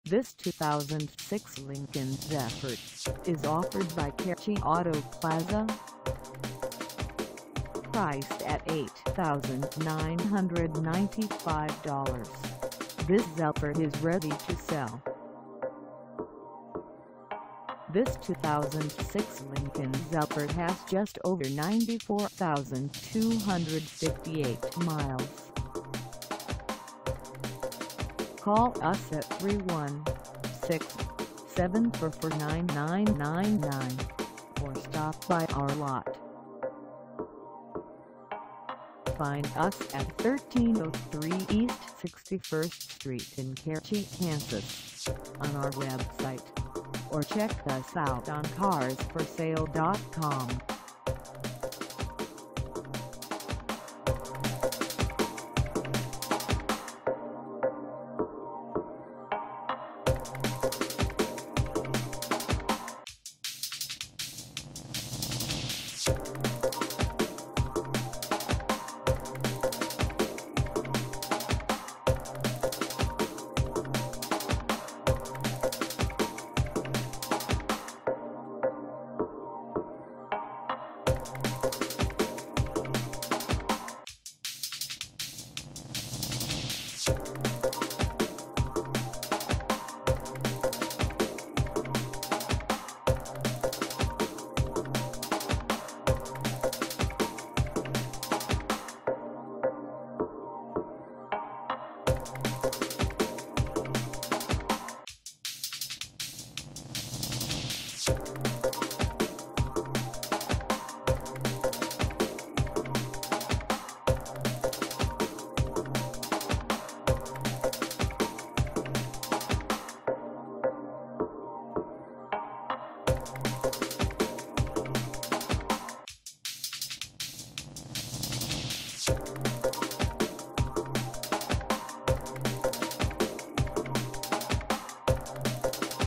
2006 Lincoln Zephyr Base for sale in KECHI, KS 67067 at the
This 2006 Lincoln Zephyr Base is for sale in KECHI, KS 67067 at KECHI AUTO PLAZA.
Engine: 6-Cylinder V6, 3.0L
Transmission: 6 Speed Automatic
Color: CREAM
Mileage: 94258
Contact KECHI AUTO PLAZA 1303 E 61ST ST KECHI, KS 67067 to test drive this 2006 Lincoln Zephyr Base today.